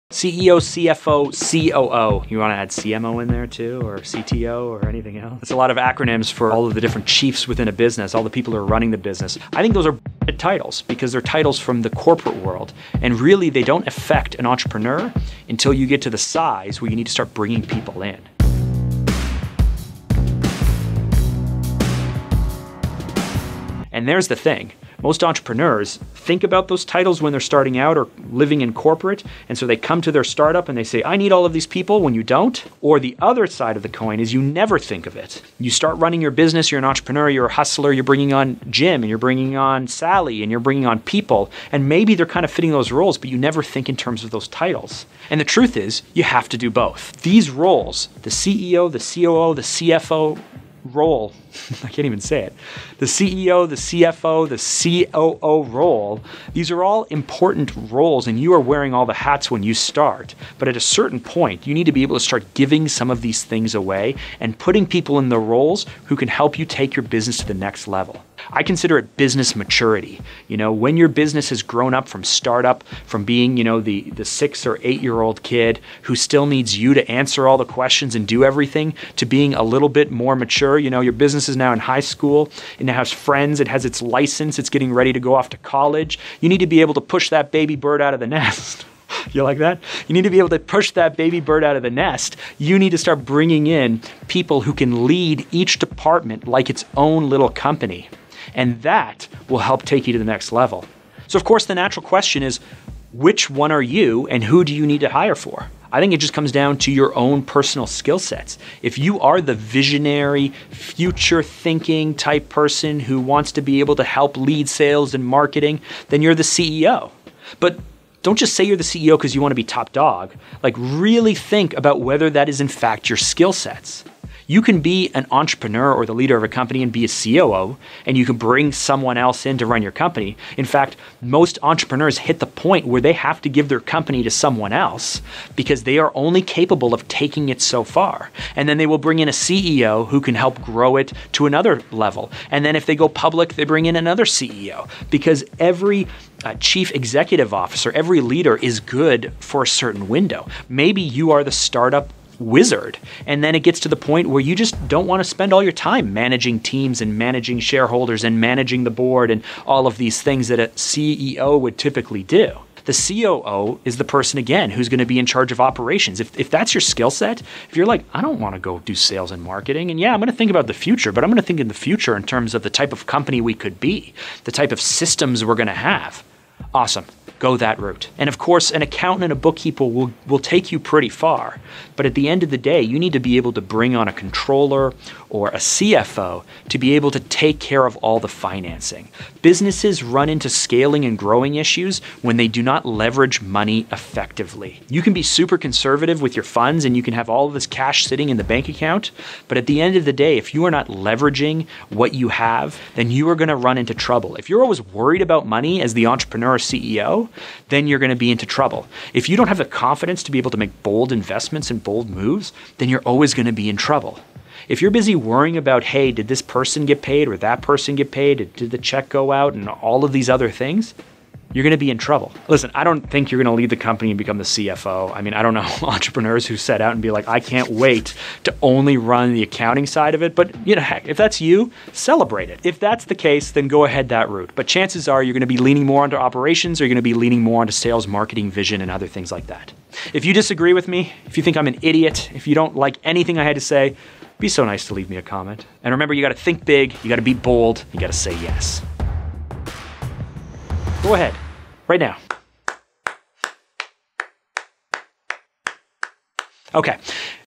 Don’t let titles get in the way of building your leadership team.
Bringing on the right people who can help you scale and grow your business, and putting them in the right roles and hiring them in the right order, is a key series of steps for every entrepreneur.  The real question of course is, what is your role and who do you hire first?